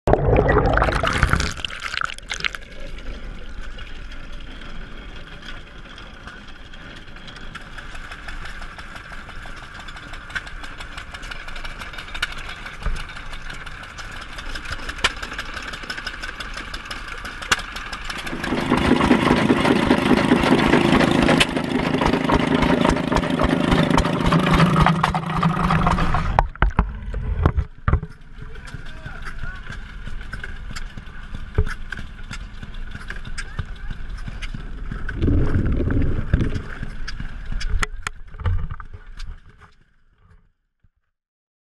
Stationary Engines in Action
Stationary engines in operation at Harvest Festival, September 5 and 6, 2015, Reynolds-Alberta Museum.